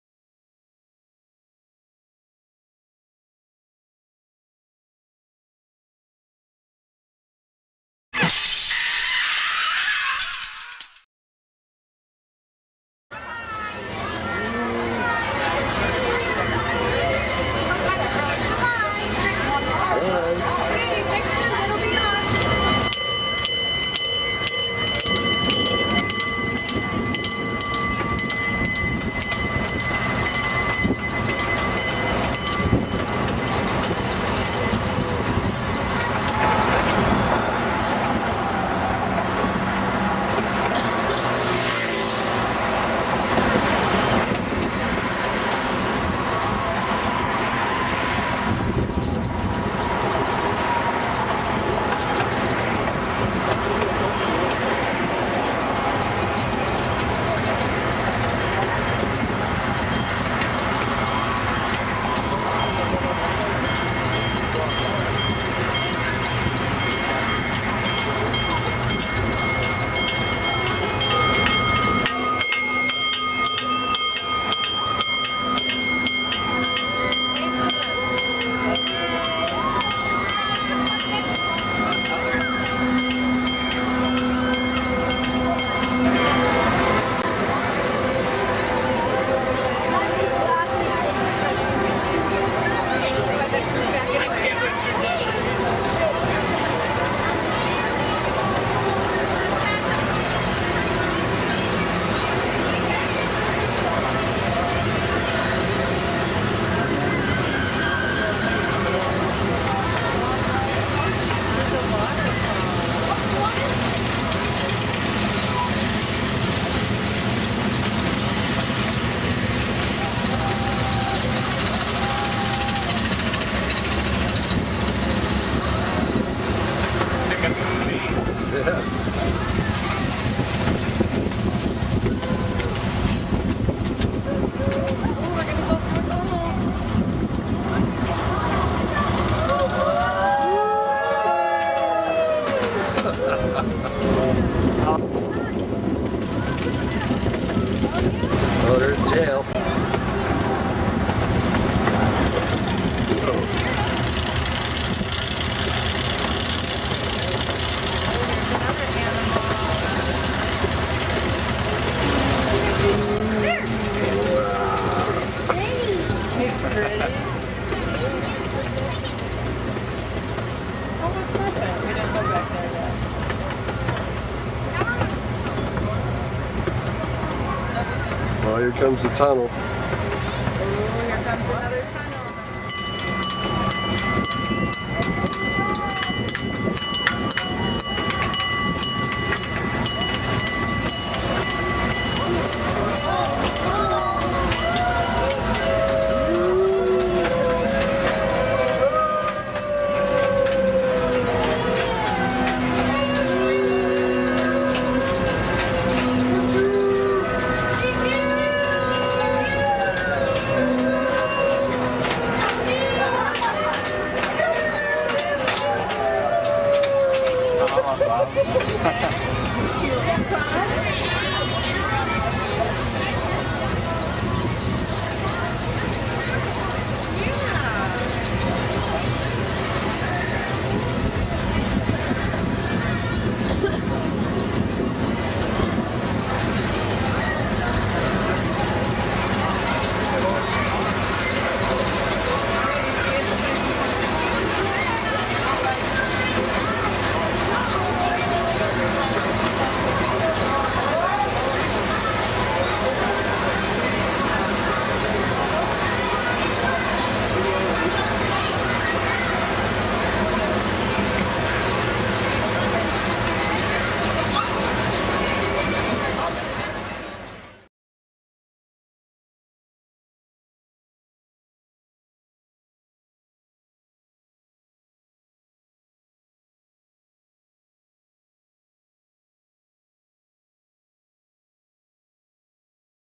Kiddieland - The Gas Train Miniature Railroad Ride 2009
Take another ride on the Gas Train Miniature Railroad Ride at Kiddieland Amusement Park in Melrose Park, Illinois. This video clip was recorded on July 3, 2009 during the final season of this park after 81 years of providing family oriented fun and entertainment. Video By Victor Modlinski. The Outernet Web